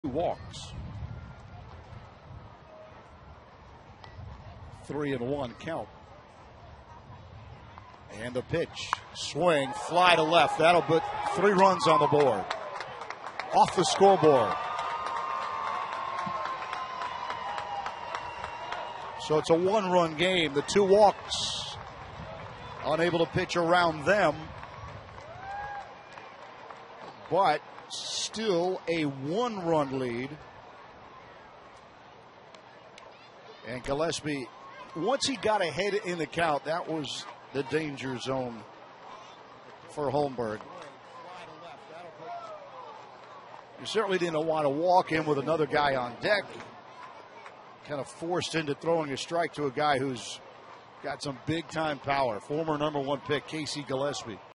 Montgomery's Casey Gillaspie hits monster shot
5/22/16: Montgomery's Casey Gillaspie rings one off of the scoreboard for his eighth home run of the season